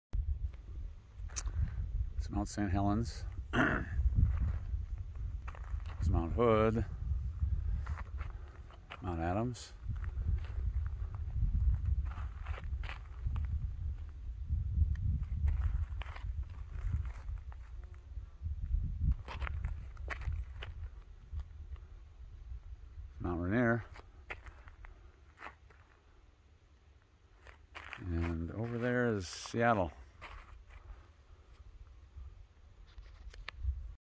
A panoramic view from the top of Elk Peak in Washington
This view is a panoramic view starting looking at Mount Saint Helen's in the west, then moving around to Mount Hood, Mount Adams around to Mount Rainier and then back to Mount Saint Helen's.